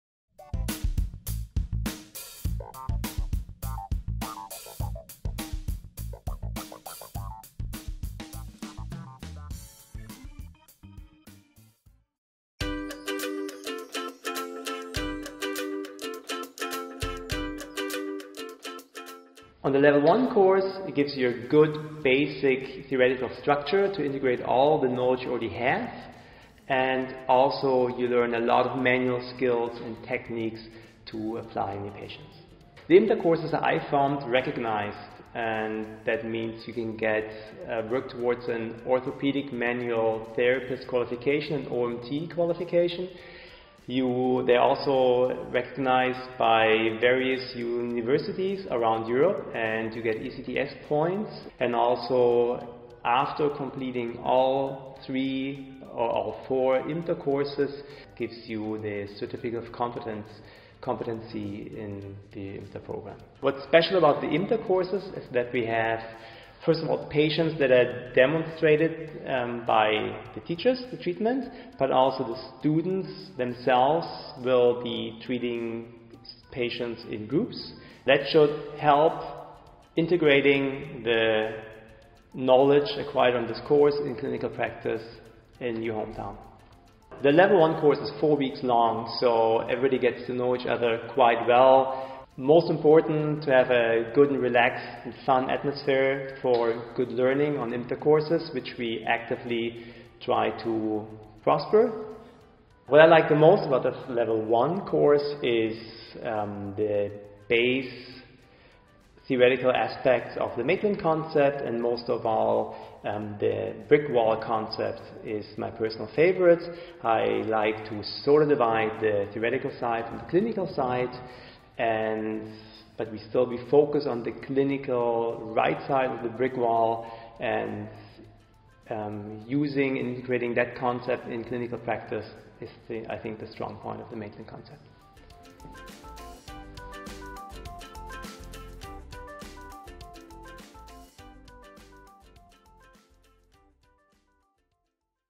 Concepto Maitland® de Fisioterapia Manual. Nivel 1. Stefan Schiller.
El objetivo general de la formación es educar a los fisioterapeutas en la resolución integral de problemas y tratamiento de los pacientes con trastornos neuromusculoesqueléticos del sistema de movimiento basado en los principios del Concepto Maitland ®.